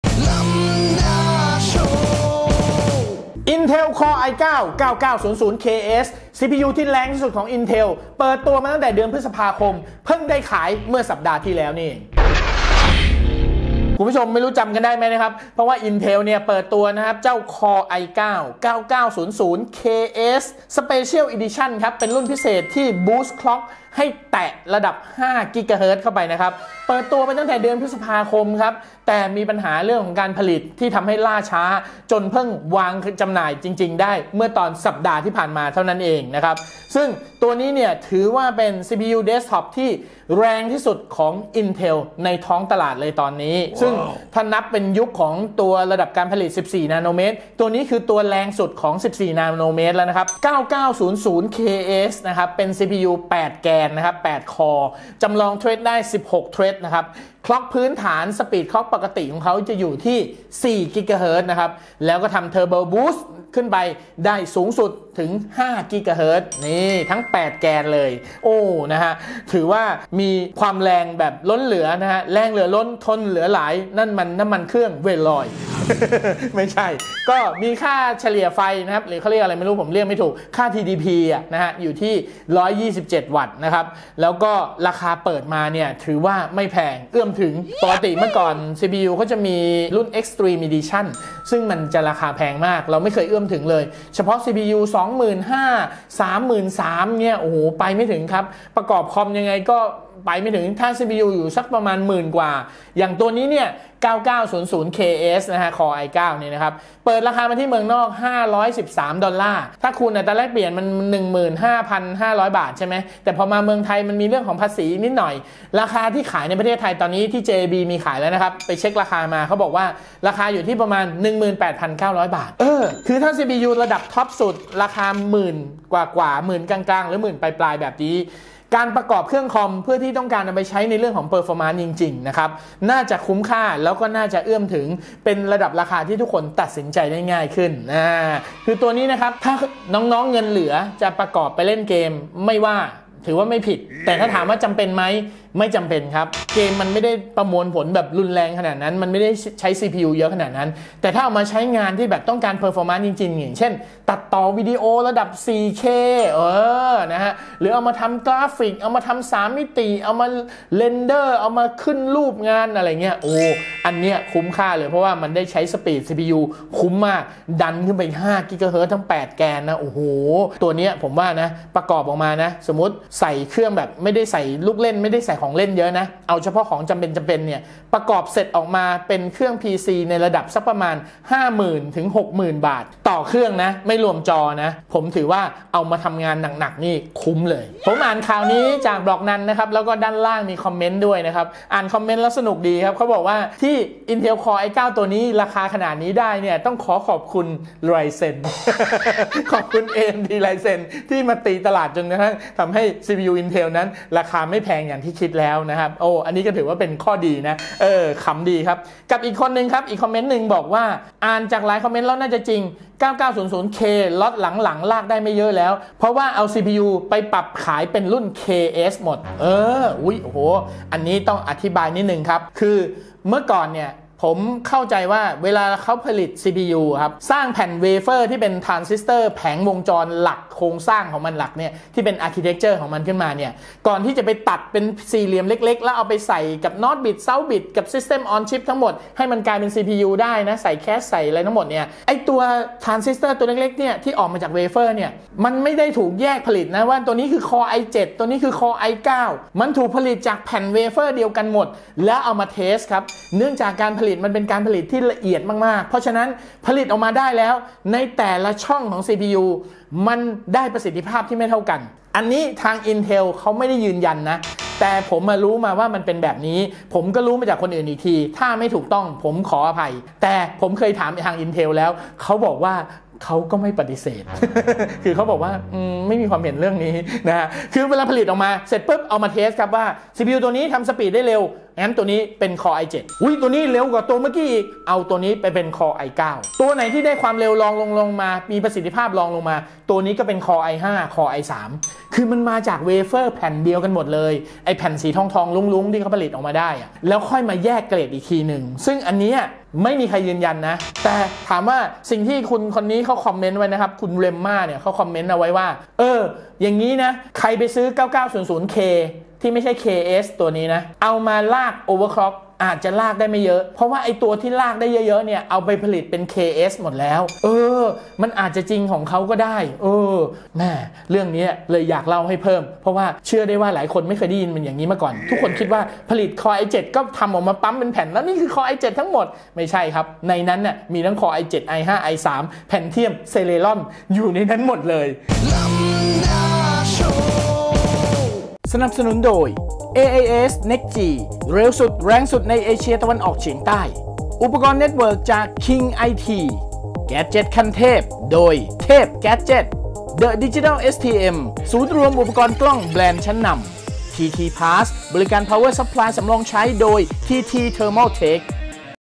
ขายแล้ว CPUที่แรงที่สุดของ Intel core i9 9900ks
ขายแล้ว CPUที่แรงที่สุดของ Intel core i9 9900ks 8 คอร์ 16 เทรด ทุกคอร์บูสสูงสุด 5 Ghz ราคาไทย 18,xxx บาท